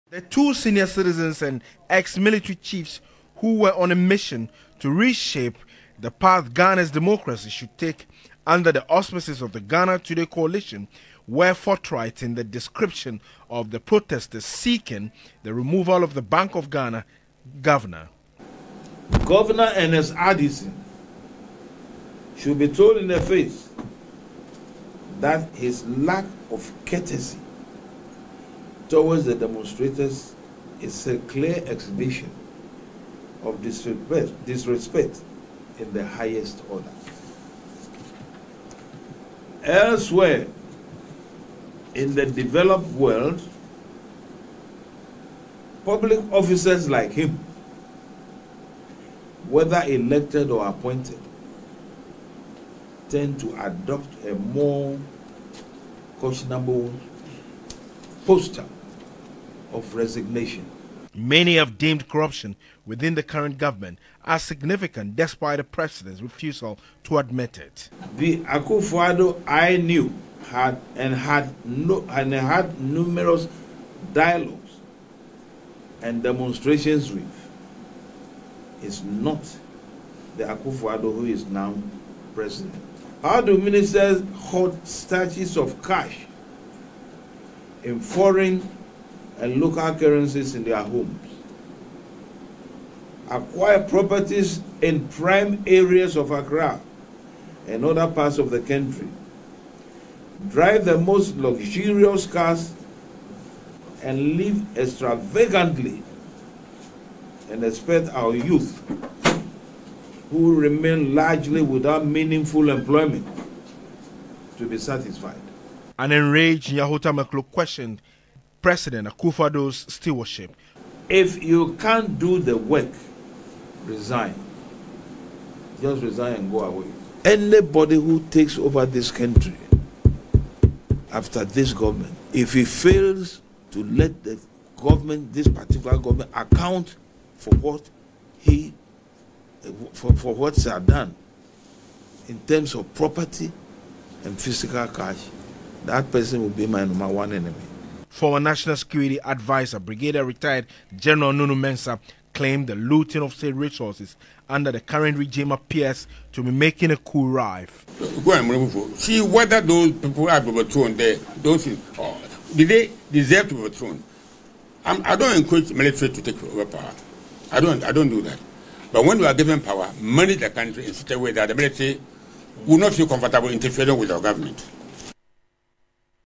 Resign if you cannot govern Ghana, ex military chiefs to President Akufo Addo
two retired military officers have strongly criticized Ghana's president, questioning his leadership style.

founding member of the New Patriotic Party,  dr. Nyaho Nyaho-Tamakloe and former national security adviser Brigadier General Nunoo Mensah have warned that President Akufo-Addo's direction is dangerous for Ghana's democracy and recommend he relinquish his presidency.